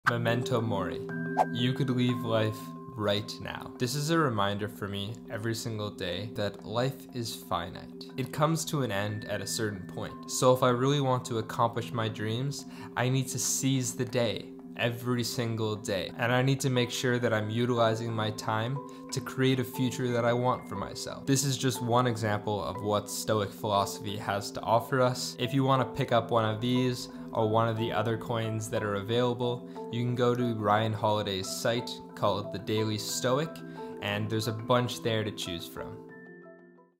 The Most Important STOIC Lesson!
Life is finite, it's a fact that we often forget or neglect. It's a reminder that we need to seize the day every day if we are going to live the life we've always wanted. Time should be cherished, instead of wasted. If we wait it will be gone before we know it.